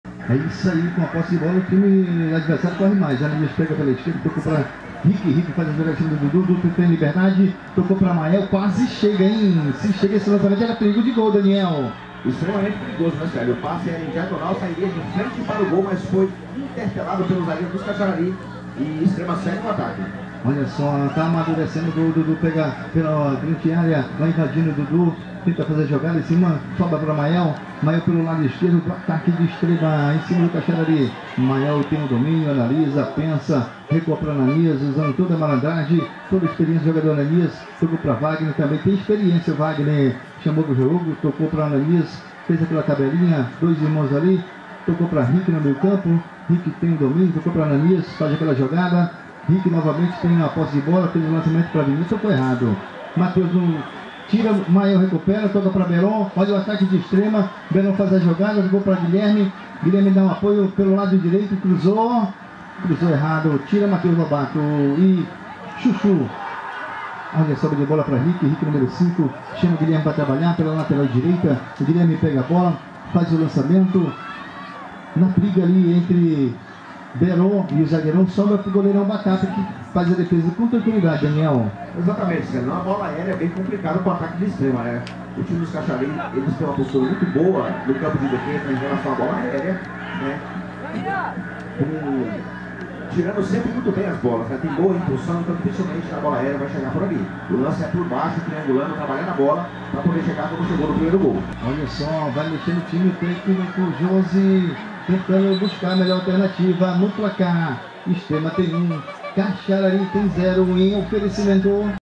Extrema x Kaxarari
Resumo da partida classificatório do Interdistrital 2022